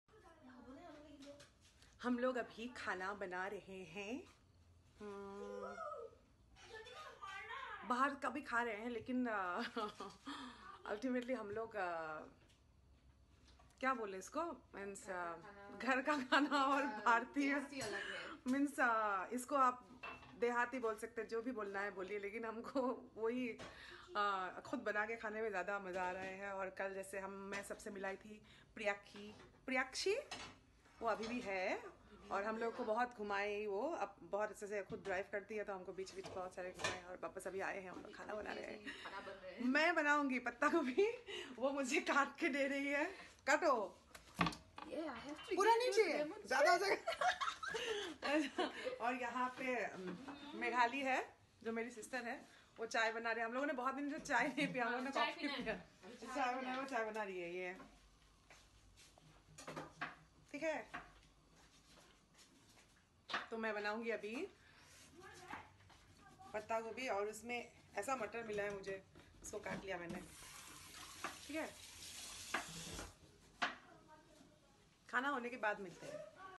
Kalpana Patowary Cooking at Brisbane Australia | Kalpana Home Maker
Just me in my favorite room of my place, the kitchen... I love cooking  and even better when I have good music...

Kalpana Patowary folk singer of India invited to perform in the Commonwealth Games 2018 in Australia.Here are a small video of her cooking in Edward in Mantra Brisbane Australia service appartment ahead of the Games.